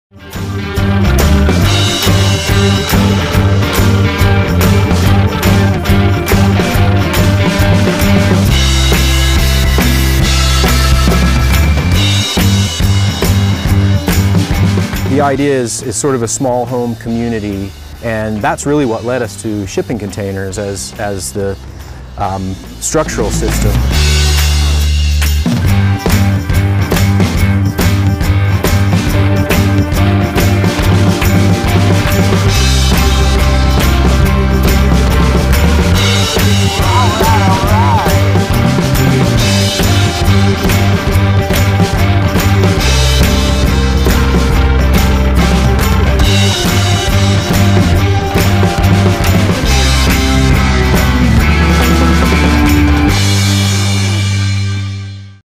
Would you live in a shipping container home?
Would you live in a home made of shipping containers? The “Luxury Shipping Container Homes” will soon be ready to lease in Oklahoma City. Watch the video to learn more.